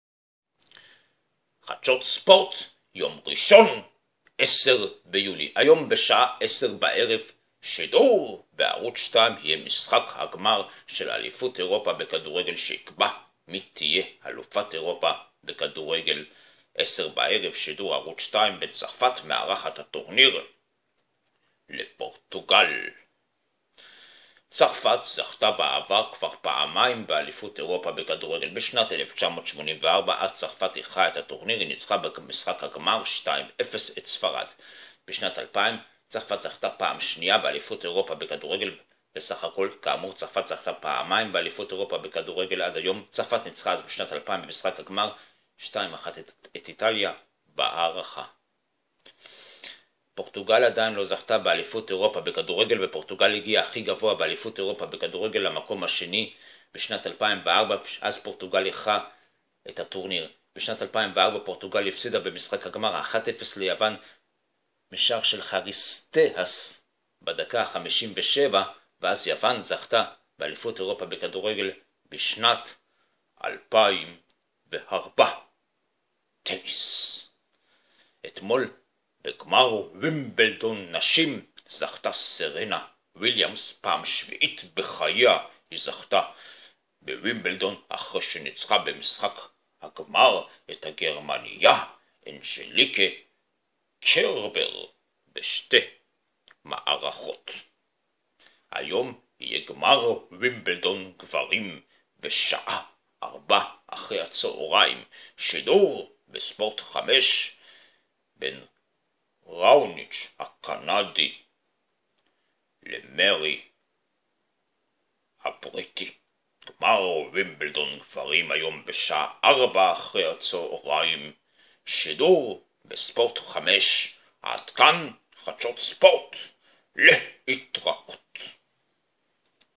יום 456 -חדשות ספורט ראשון 10/07/16 - צרפת מול פורטוגל - משחק גמר אליפות אירופה
פורטוגל אלופת אירופה
צרפת 0  פורטוגל 1   אדר  דקה    109
רונאלדו  הוחלף בעקבות פציעה דקה 25

משה דמאיו הוא מנכ"ל בית"ר ירושלים

קבצותו של זהבי ניצחה 2:0 בליגה הסינית, זהבי נכנס מחליף דקה 62 והיה מעורב בשער השני

חביי  0 גוואנגז'ו  ר.פ  2
מארי  אלוף ווימבלדון , מארי הבריטי ניצח את  ראוניץ' הקנדי  ב 3 מערכות
 אתמול סרינה  ויליאמס זכתה פעם שביעית בחייה  ביחידים נשים בווימבלדון

חנה קנייזבה מיננקו  קפיצה משושלת מדלית   כסף אליפות אירופה